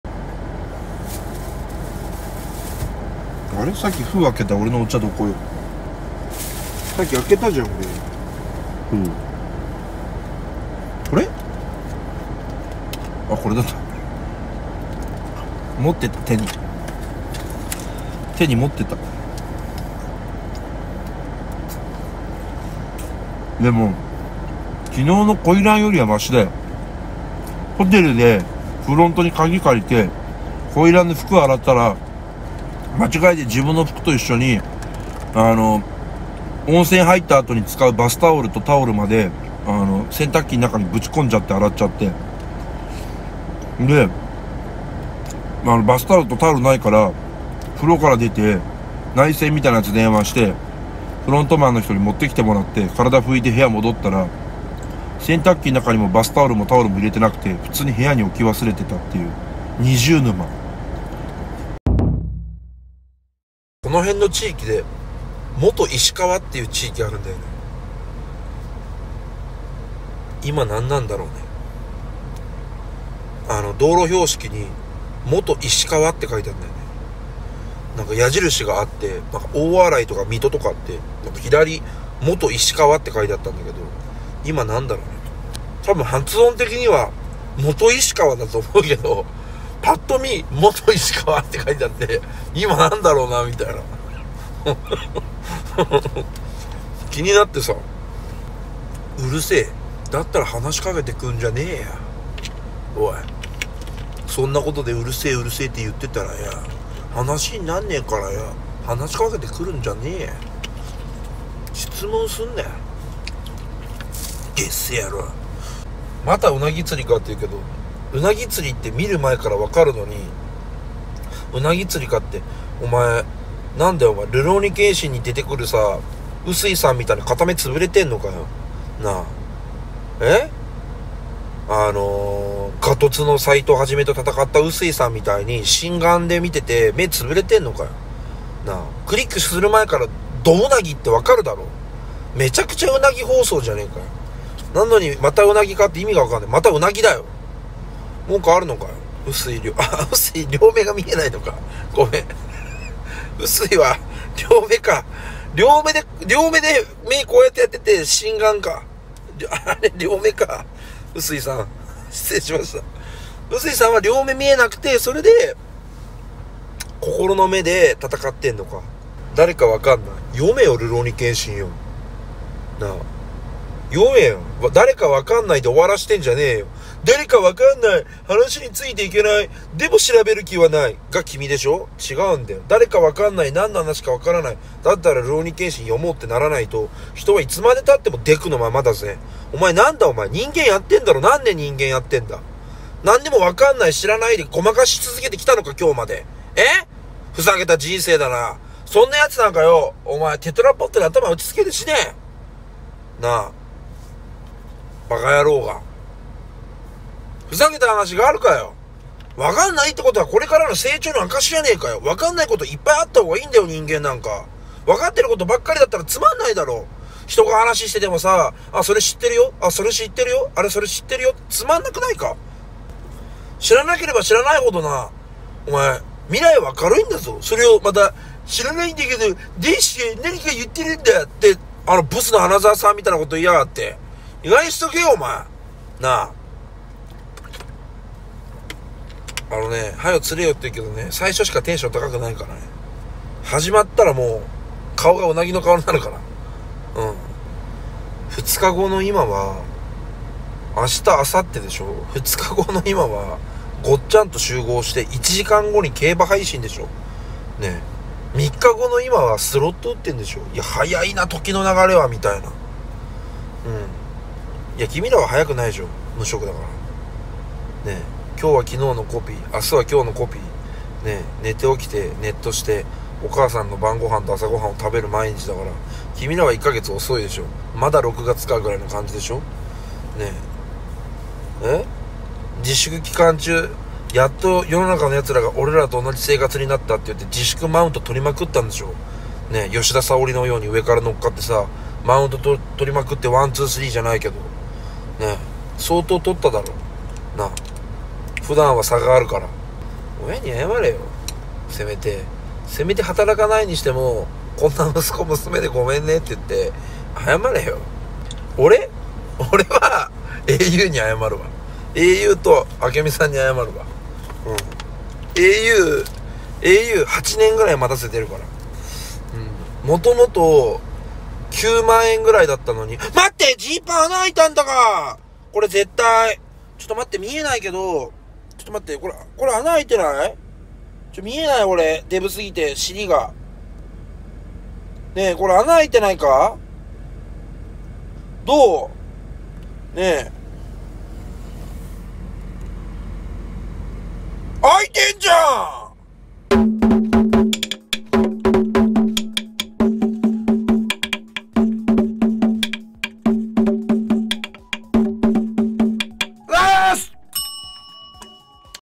休憩中にリスナーを煽ってたら天罰がくだる沼男【鰻釣り配信】
放送日　2020/06/25　ガチ初心者　ひとり鰻釣り

【ノリラジ会員限定配信を見れるFRESHチャンネル】